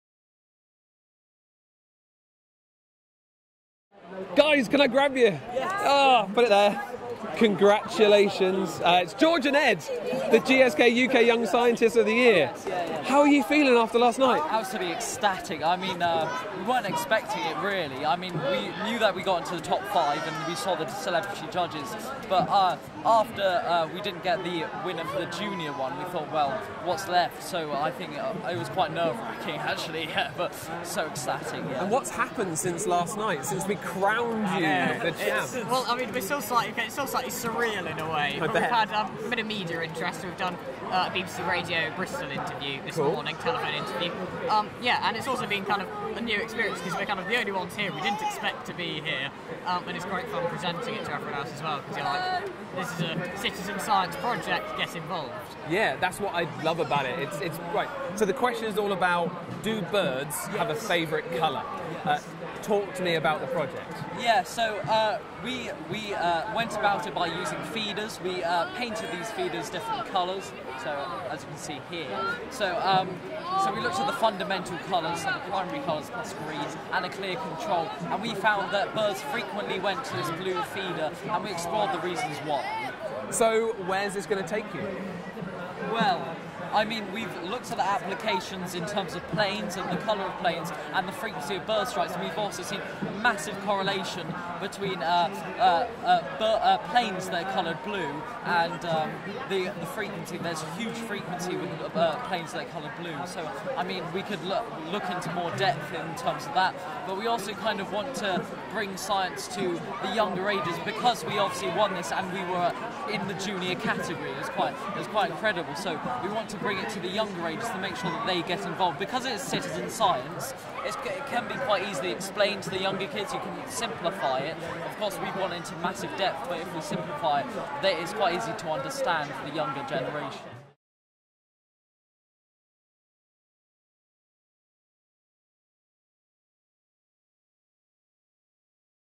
Greg Foot meet the GSK UK Young Scientists of the Year 2017
Greg Foot, out about on the Big Bang Fair show floor, meets the brilliant winners in the Big Bang Competition - here he speaks to the GSK UK Young Scientists of the Year George Rabin and Ed Thurlow as they talk about their winning project. What IS a bird's favourite colour?